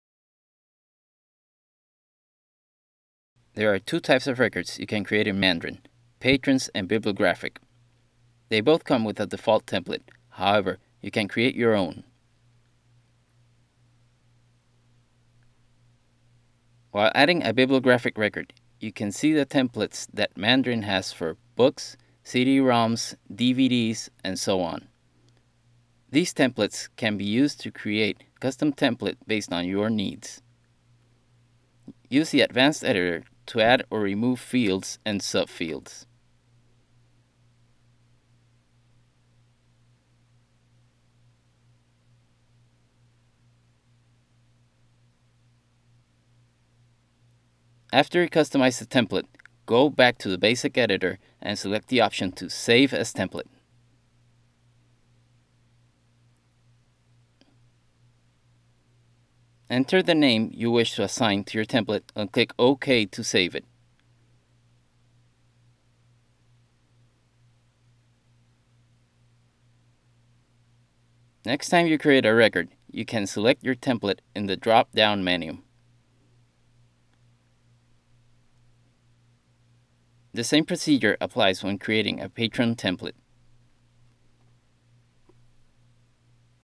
M5 Record Templates
How to edit and save record templates in m5.